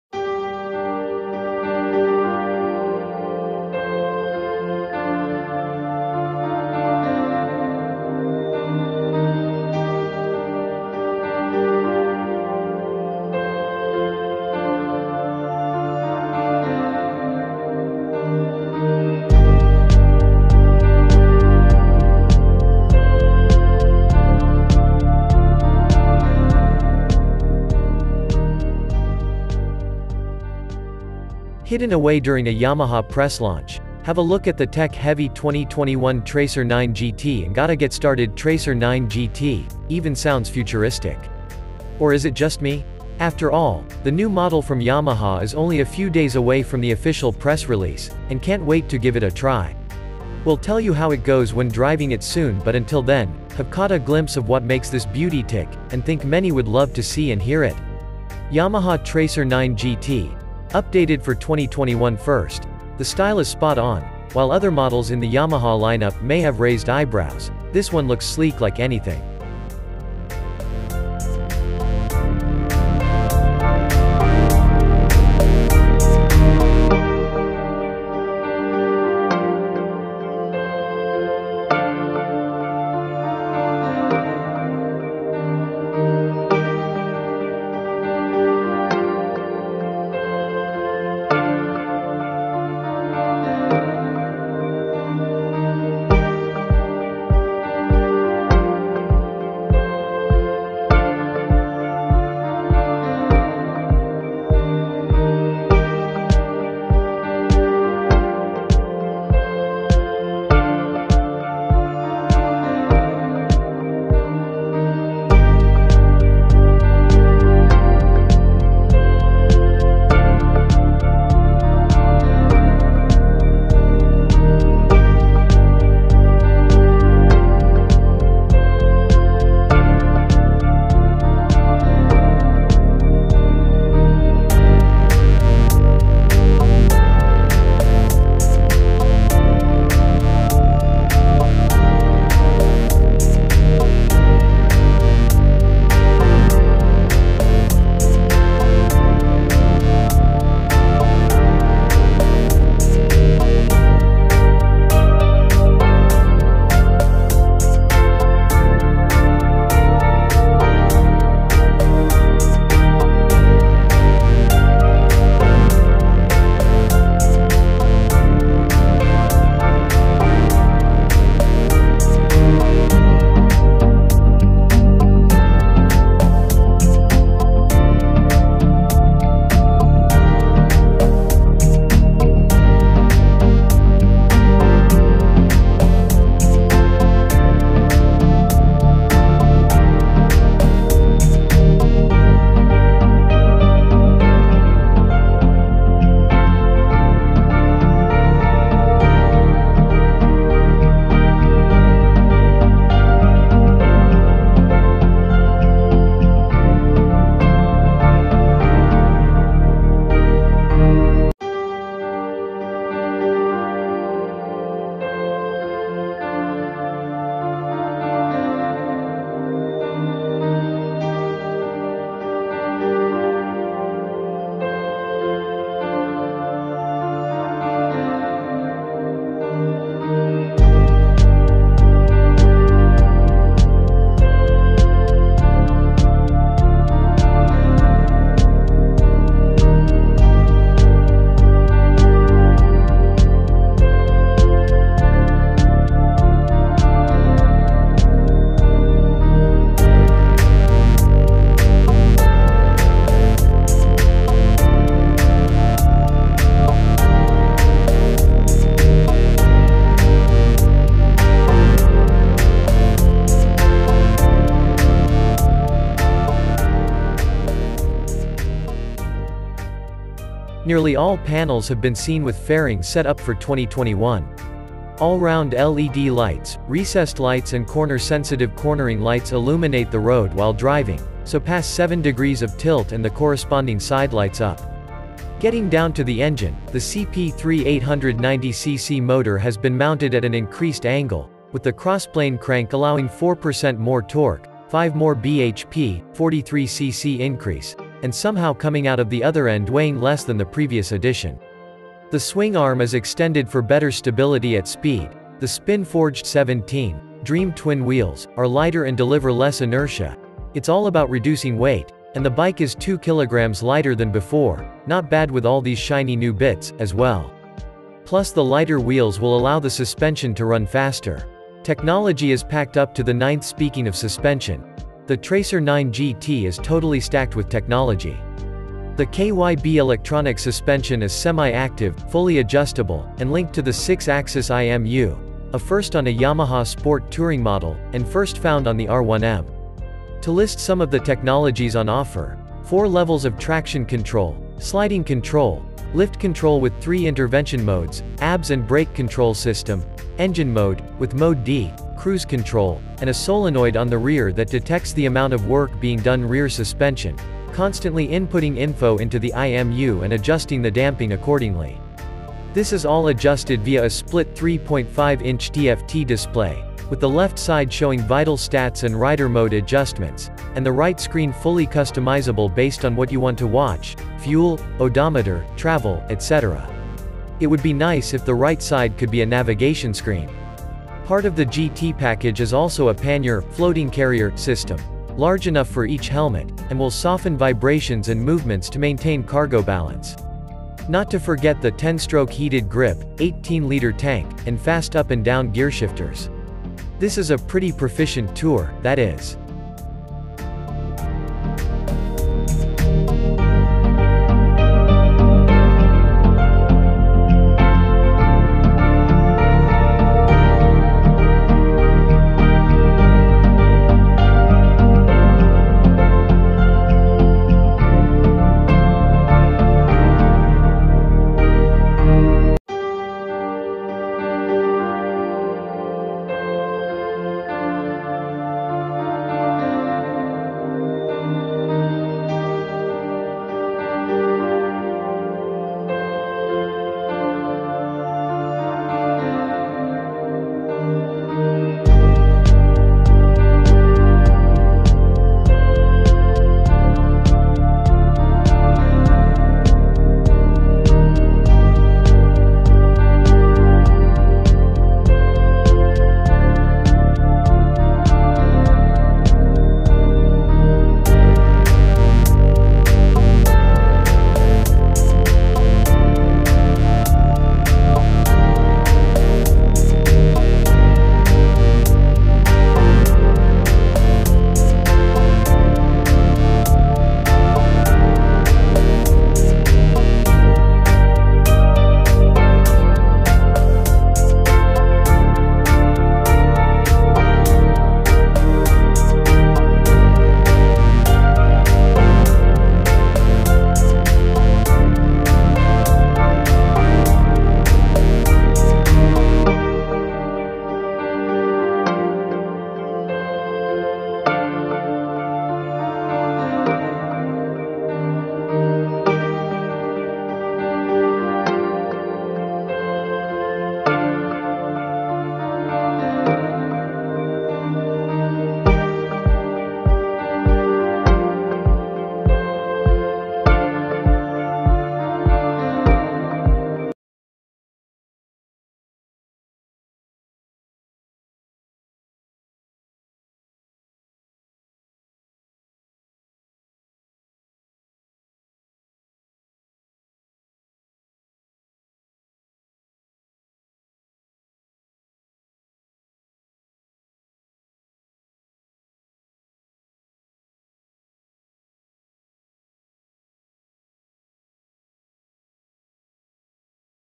The 2021 Yamaha Tracer 9 GT. There’s no room for zeros here
Yamaha Tracer 9 GT - updated for 2021
  First, the style is spot on, while other models in the Yamaha lineup may have raised eyebrows, this one looks sleek like anything.  Nearly all panels have been seen with fairings set up for 2021. All round LED lights, recessed lights and corner-sensitive cornering lights illuminate the road while driving, so pass 7 degrees of tilt and the corresponding side lights up.

  Getting down to the engine, the CP3 890cc motor has been mounted at an increased angle, with the Crossplane crank allowing 4% more torque, 5 more BHP, 43cc increase, and somehow coming out of the other end weighing less than the previous edition.